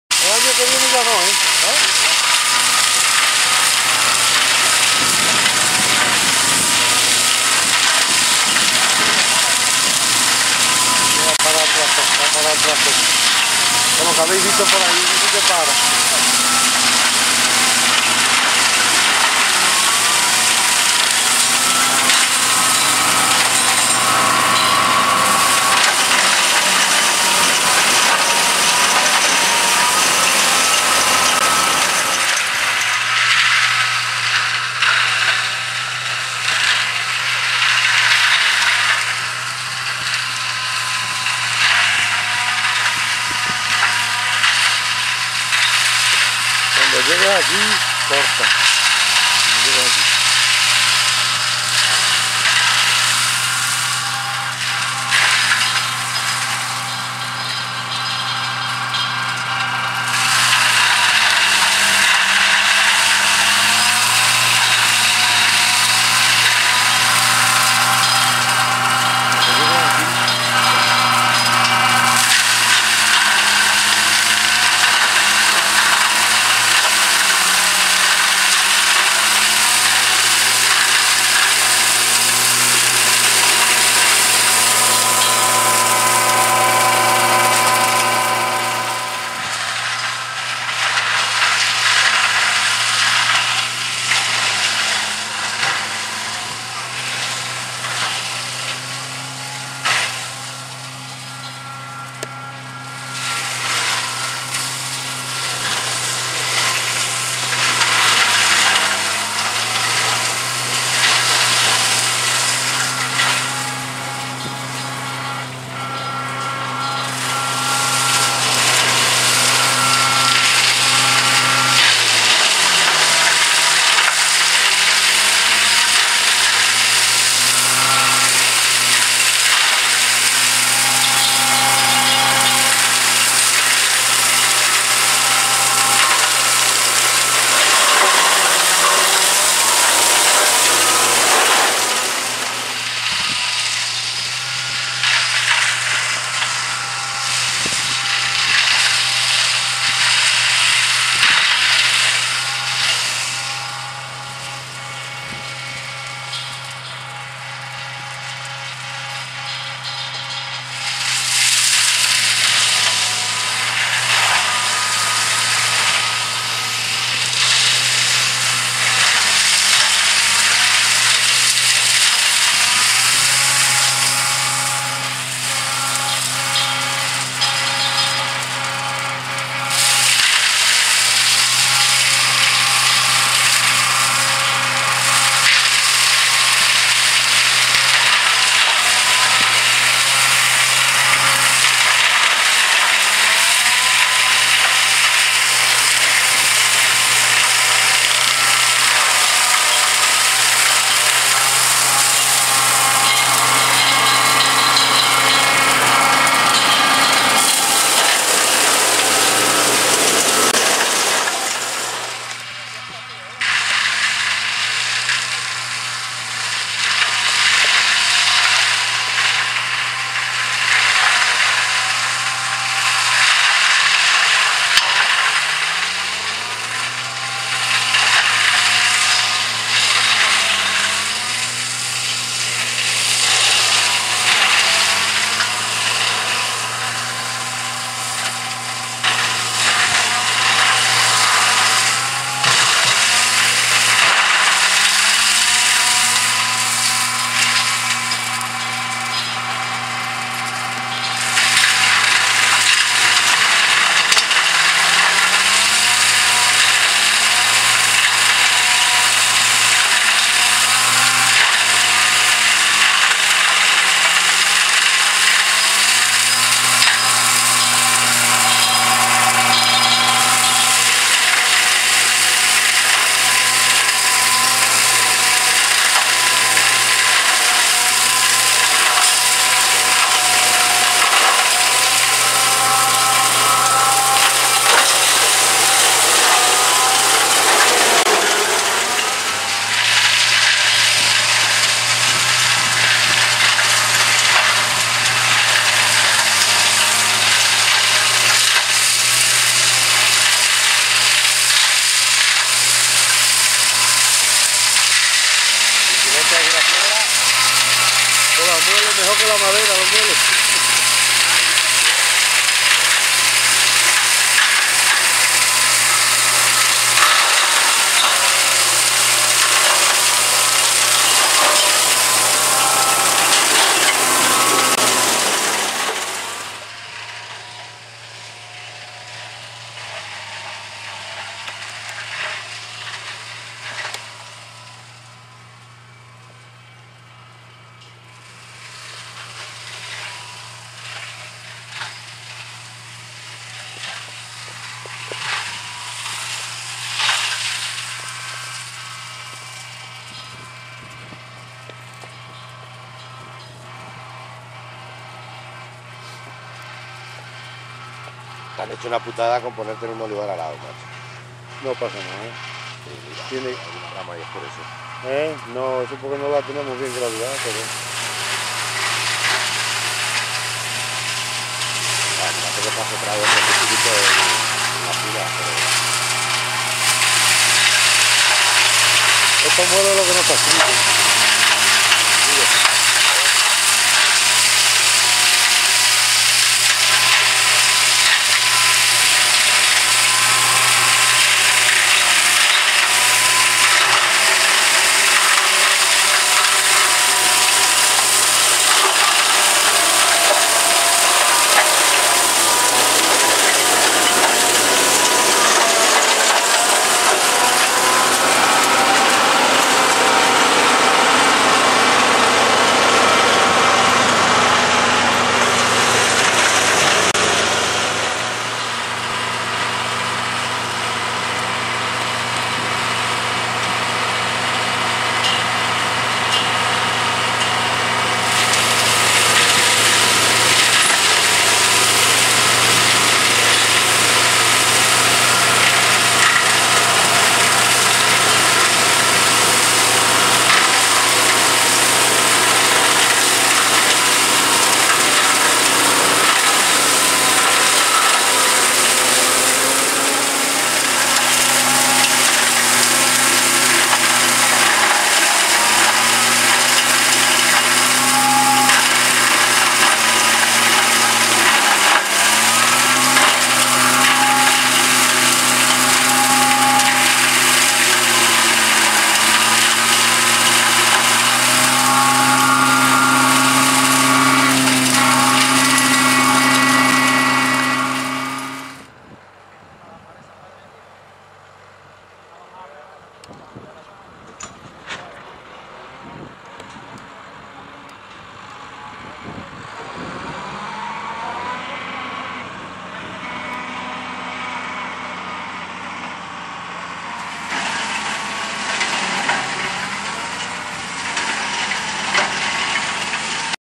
TRITURADORA REVERSIBLE TRO-G-2000 trabajando a 4 Km/hora
Triturador reversible marca GENERAL modelo TRO-G-2000-3H con alimentador hidraulico de velocidad variable, torreta de enganche con sistema reversible trabajando a 4 kilometros por hora. Para mas informacion visitar